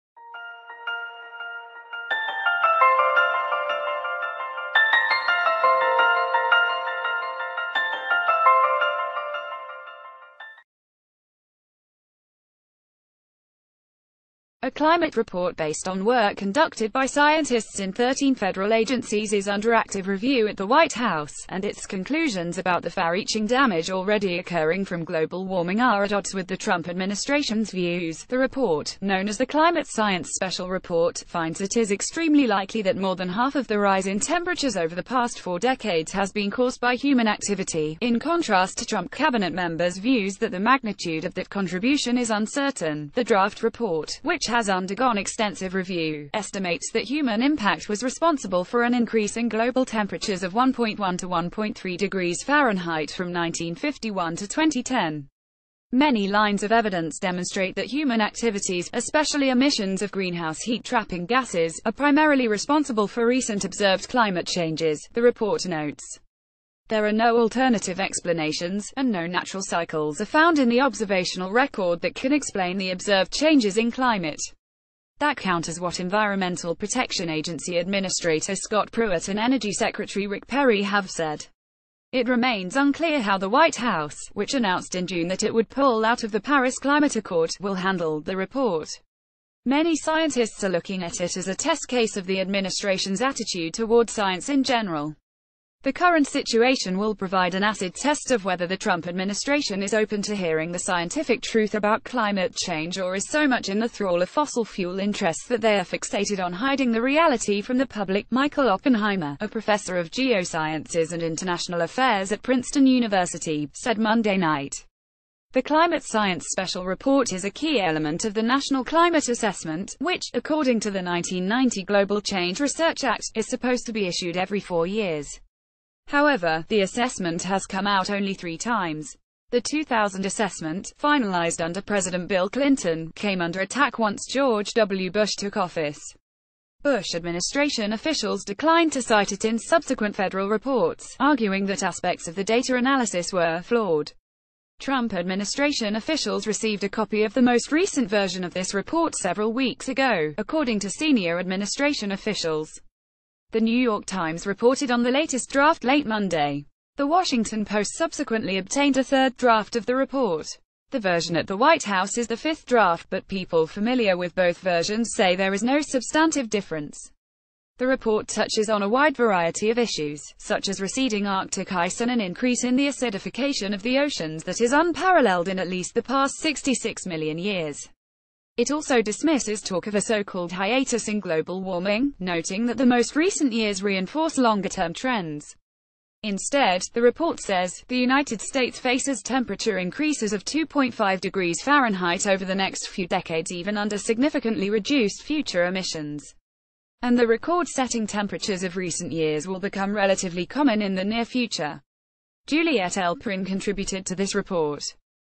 Top News | White house reviewing new report that finds strong link between climate change, human ac
White house reviewing new report that finds strong link between climate change, human activity